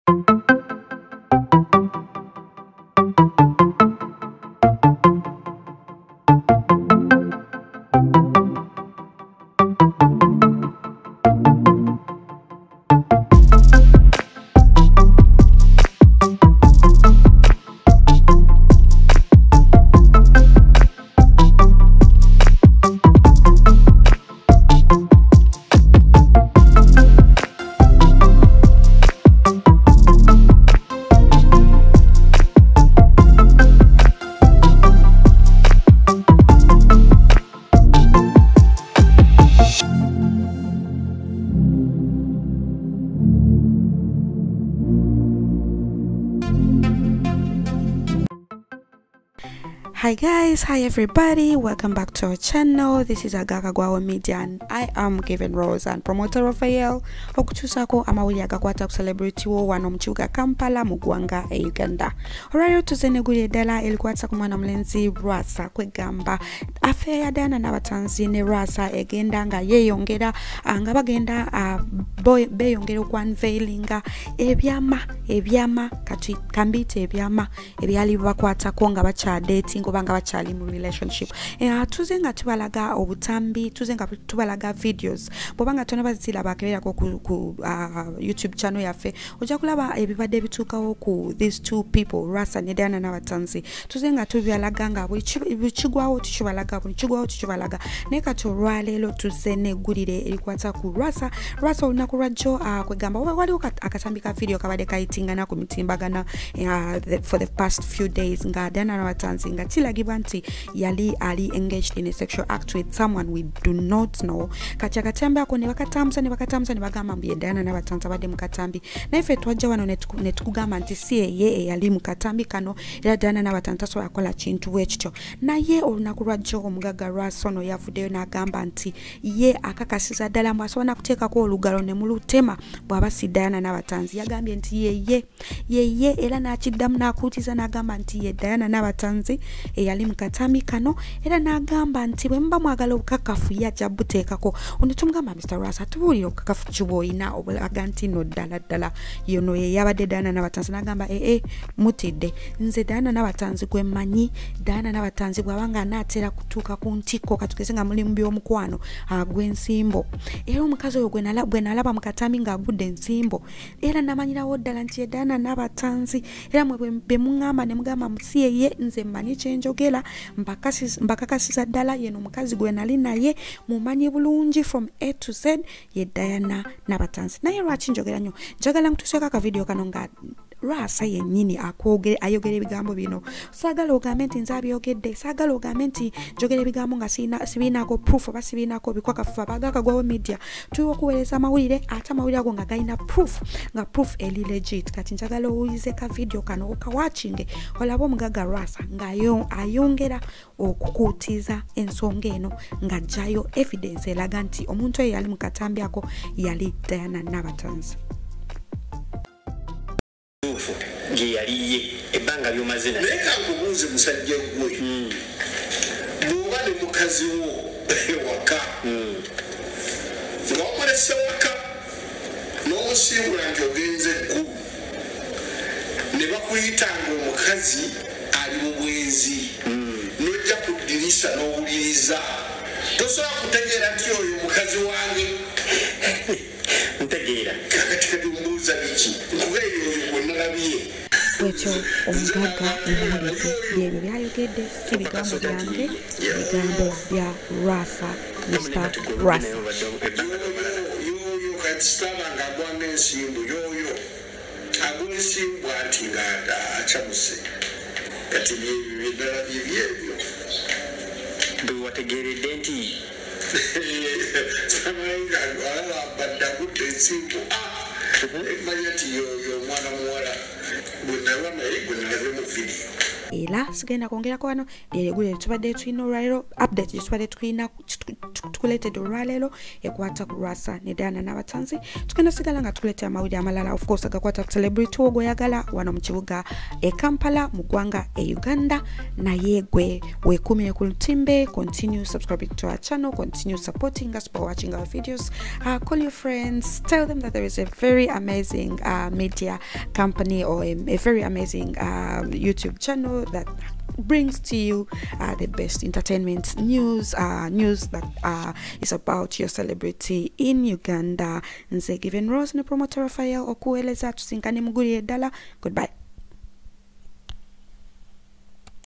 Akatambi Ka Dianah Nabatanzi Kabizadde, Lwasa Ayogedde Ebitiisa
Ugandan Taycoon and Socialite Lwasacontinues to sting Diana Nabatanzi in this video.
Please watch till the end to see new accusations from Lwasa
About Us:

Thanks For Your Support 🙏🙏 Your One Sub Means A lot To Us.